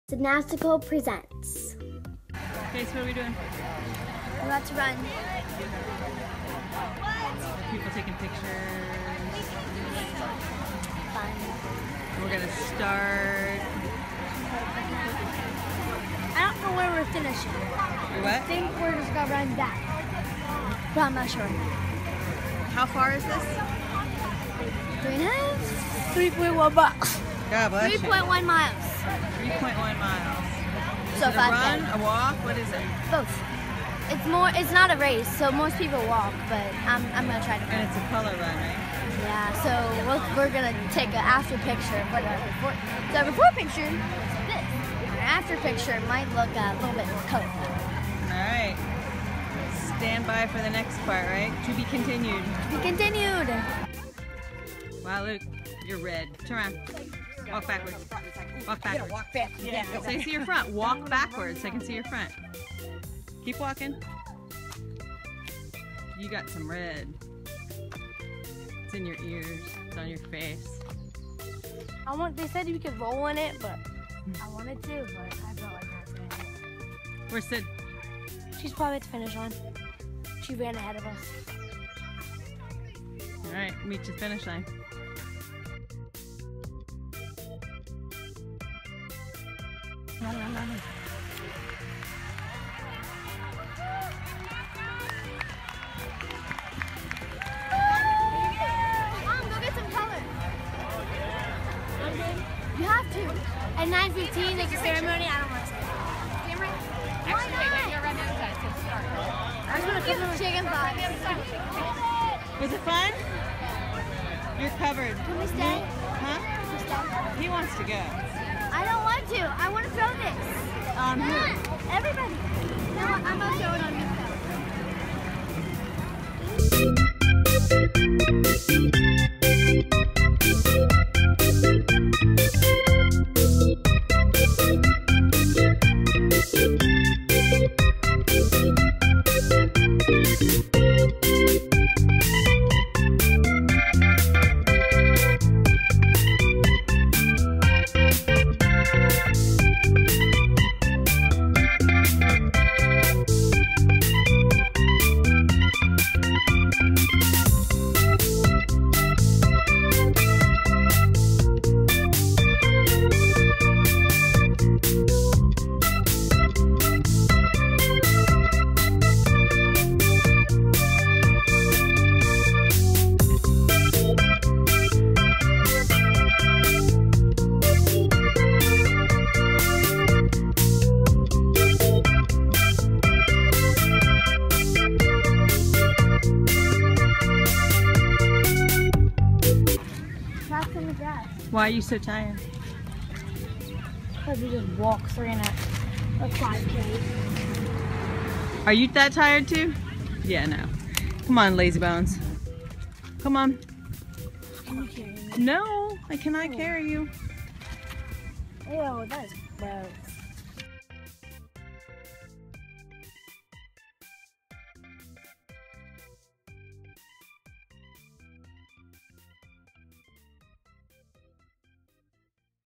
Color Run! | Sydnastical Presents!
Watch Sydney & Luke get Colored!!

All proceeds from this video are donated to the charity that you help Sydney choose.  Thanks for being a part of this just by watching her videos!